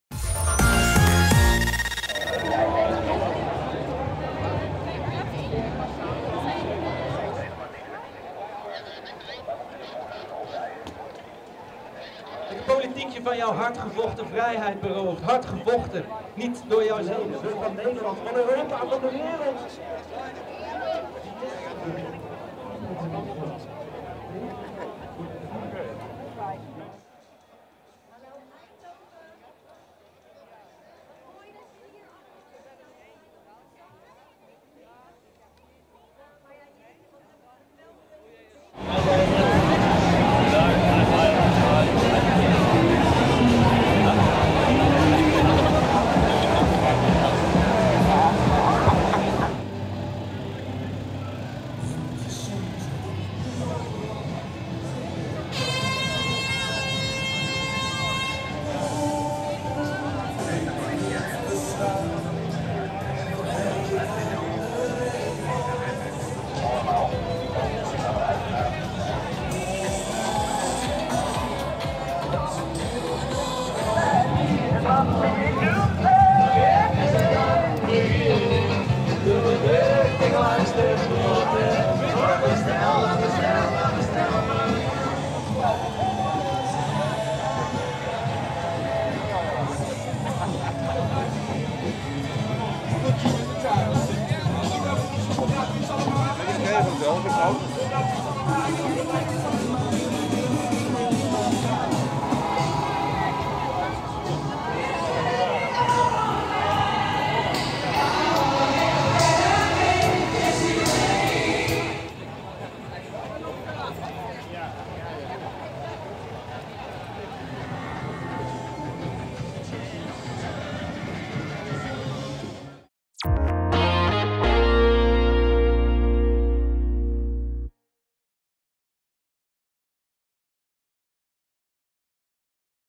Demonstranten dragen boerenzakdoek tijdens protest tegen stikstofplannen
Honderden tot zo'n duizend mensen doen zondagmiddag in Eindhoven mee aan een demonstratie van protestbeweging ‘Samen voor Nederland’. Ze protesteren tegen onder meer het stikstofbeleid van het kabinet. Uit solidariteit met de protesterende boeren dragen veel demonstranten een rode boerenzakdoek.